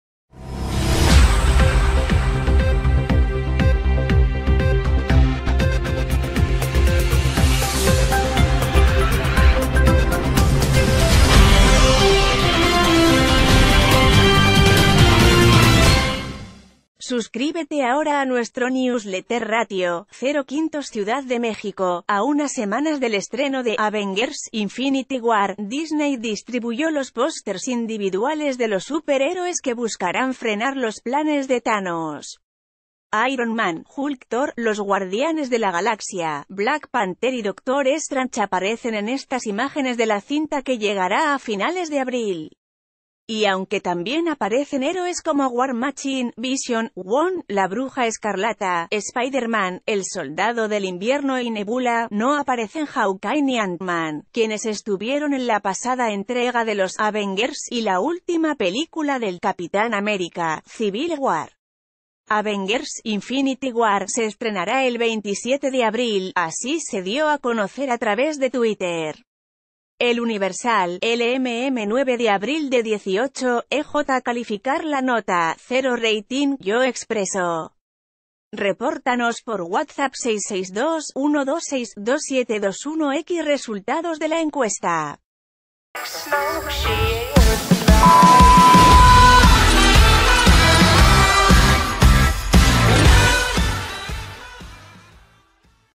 Avengers lanza posters individuales de los personajes
Suscríbete ahora a nuestro newsletter Ratio: 0 / 5 CIUDAD DE MÉXICO.- A unas semanas del estreno de "Avengers: Infinity War", Disney distribuyó los posters individuales de los súper héroes que buscarán frenar los planes de Thanos. Iron Man, Hulk, Thor, Los Guardianes de la Galaxia, Black Panther y D...

{hashtag}